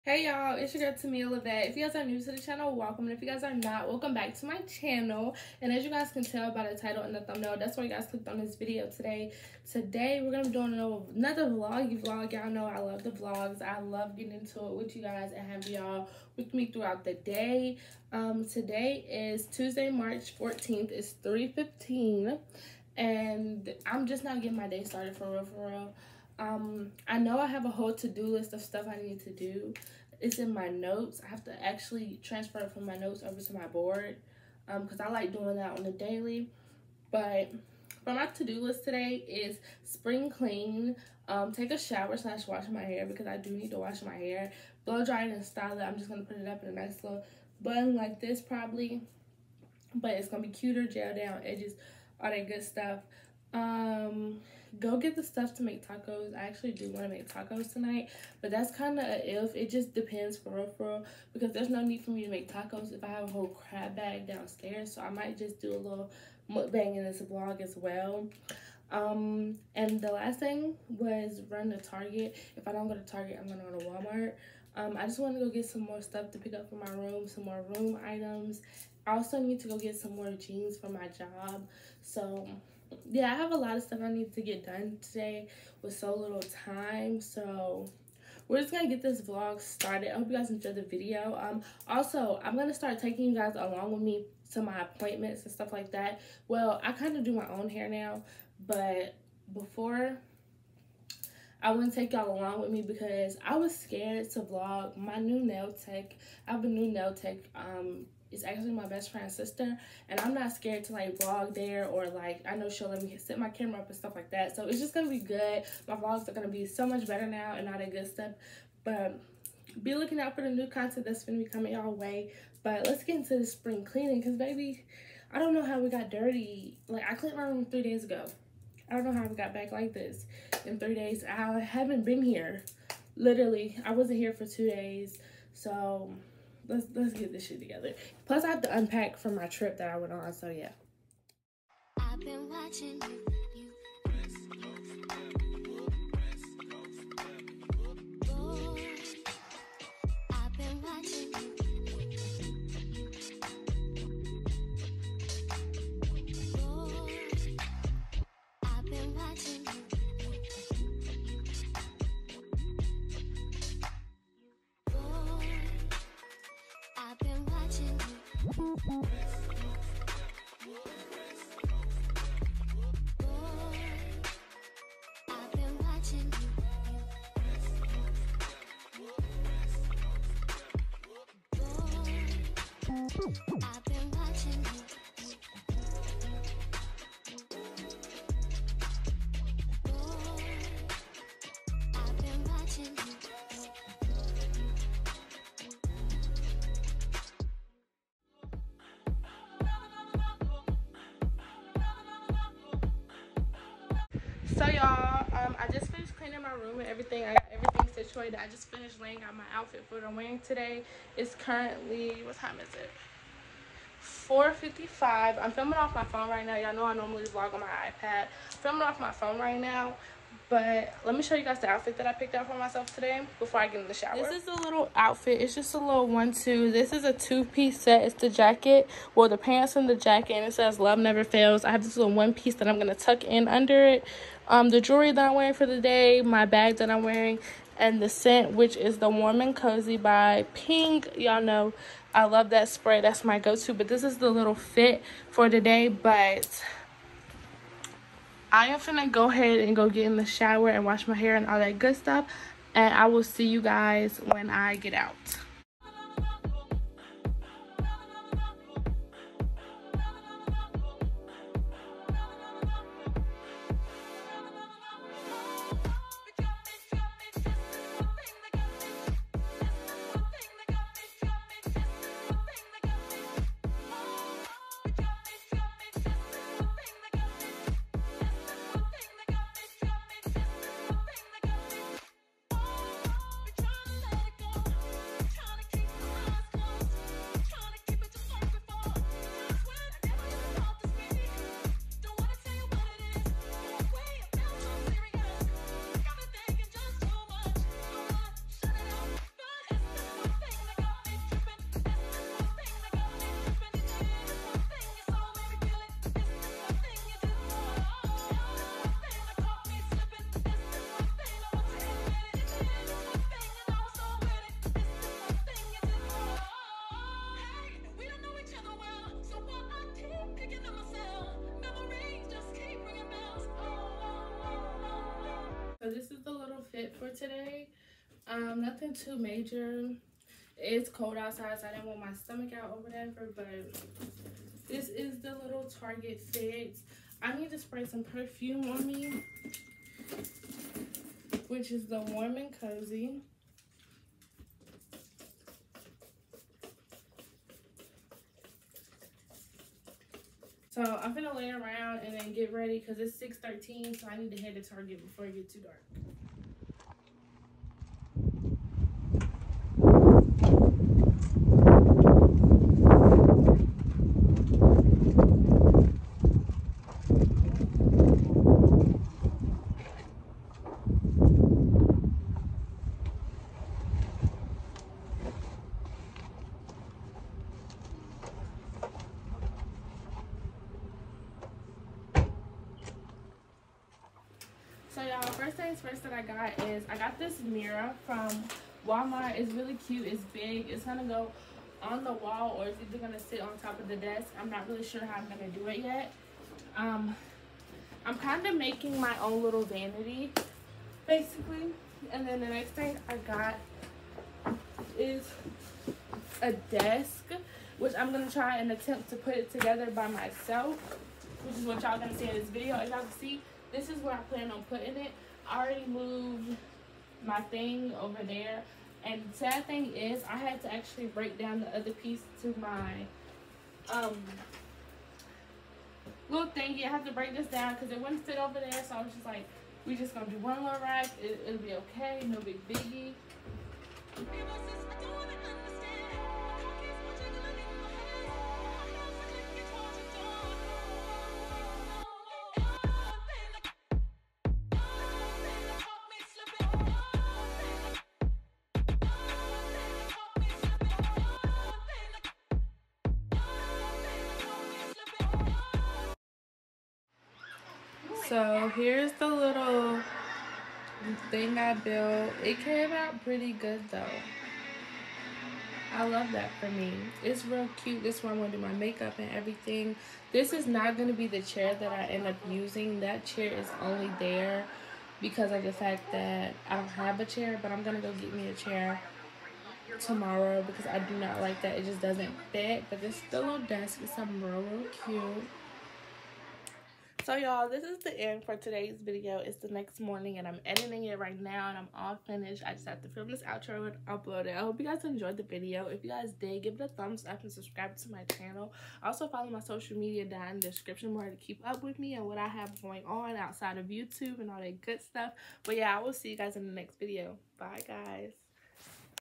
KEEPING UP WITH TAMIA LEVETTE | spring cleaning, wash day, new desk, etc..
Get to know me!
Name? Tamia
Age? 18
Film with? iPhone 12 Pro Max ’